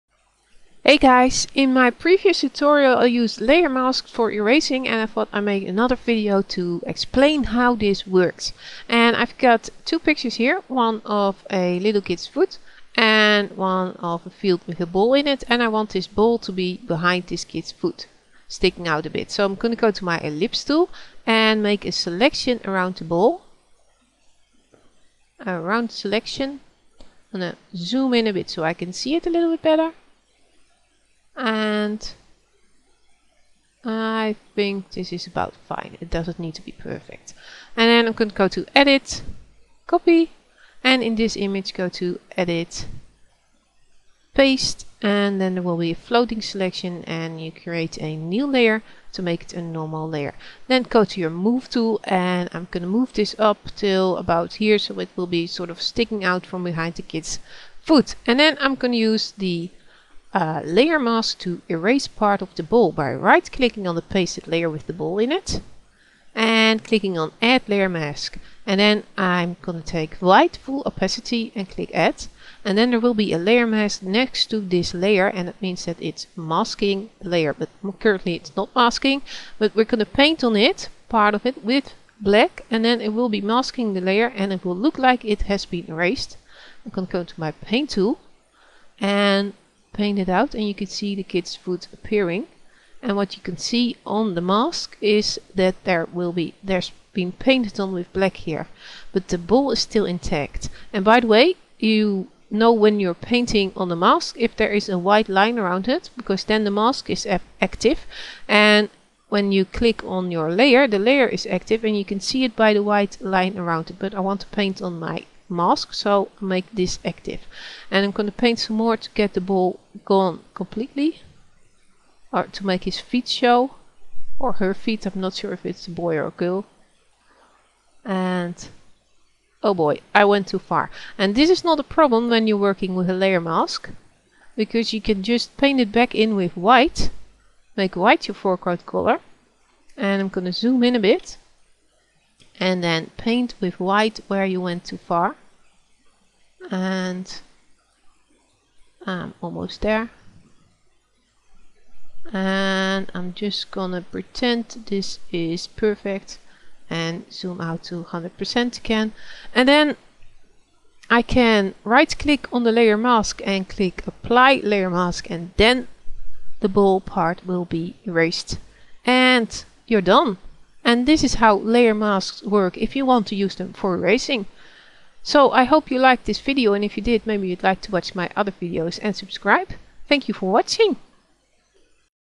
GIMP tutorial -Layer mask as Eraser Tool (fully explained)
In this Gimp tutorial I'll be showing in depth how to user a layer mask to erase part of a picture. This method is used because it is non destructive editing.

Credit:
Both images taken from BurningWell.org
Foot image owner:Leon Brooks
Golfball image owner: Luke